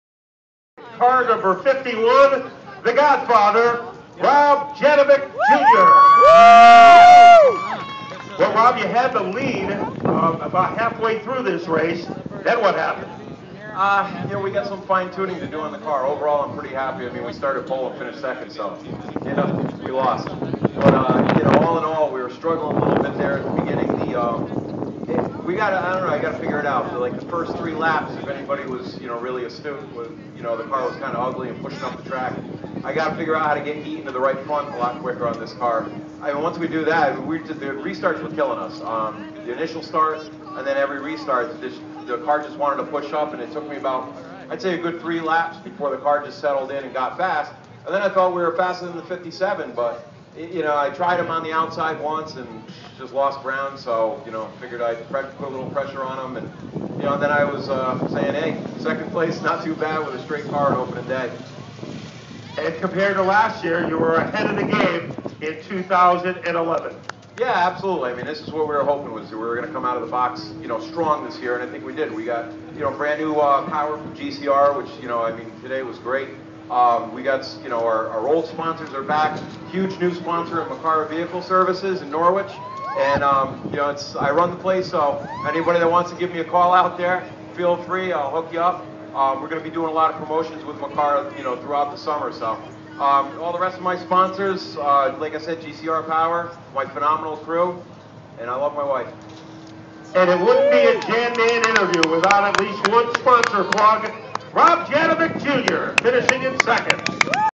April 3, 2011 Season Opener 2nd Place Finish
61 Lap feature to begin the 2011 NASCAR Weekly Racing Series at the Waterford Speedbowl